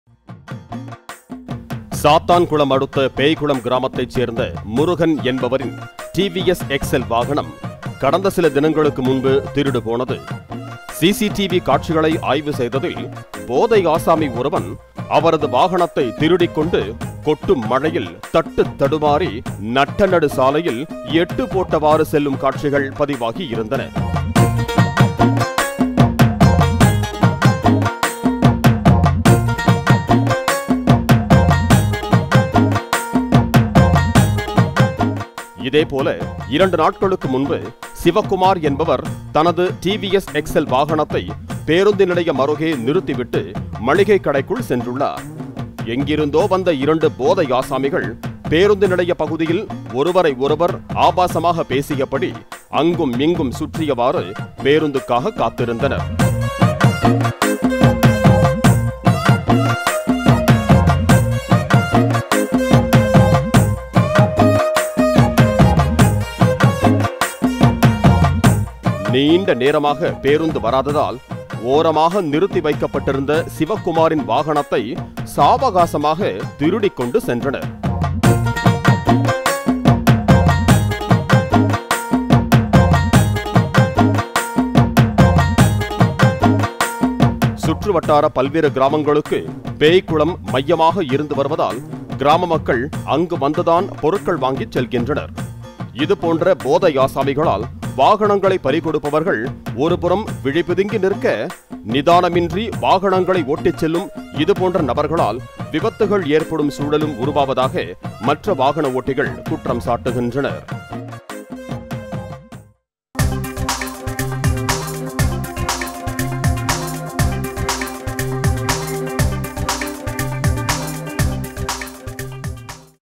"குடிகார" திருடர்களின் அட்டகாசம் - நட்டநடு சாலையில் "எட்டு" போட்டு சாகசம்..!
கேள்விக்குறியாகும் சாலைப் பாதுகாப்பு - பீதியடையும் வாகன ஓட்டிகள்

 உரிய நடவடிக்கை எடுக்க கோரிக்கை